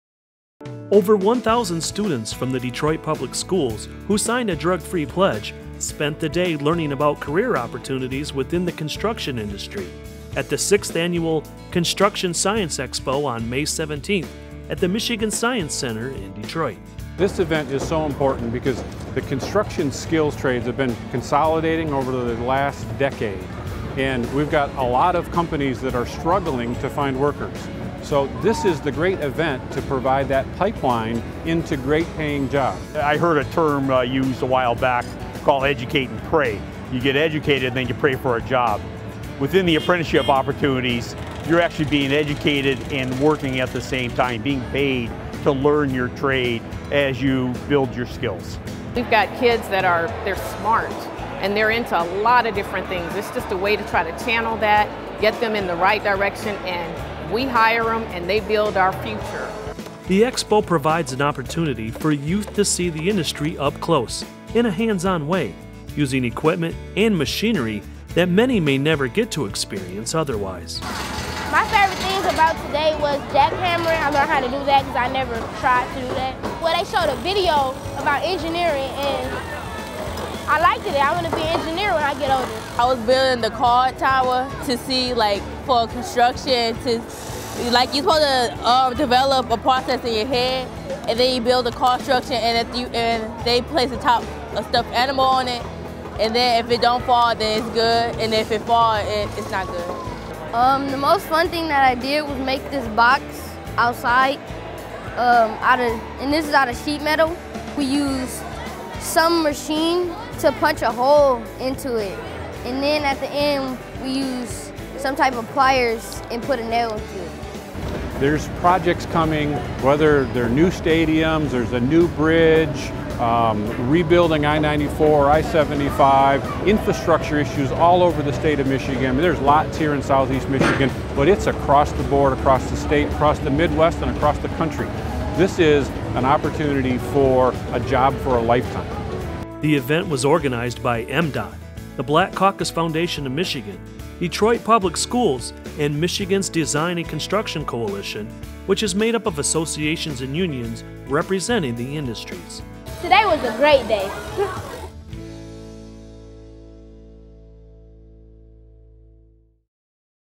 Construction Science Expo 2016
More than 1,000 students from the Detroit Public Schools who signed a drug-free pledge spent the day learning about career opportunities within the construction industry at the 6th Annual Construction Science Expo on May 17 at the Michigan Science Center in Detroit.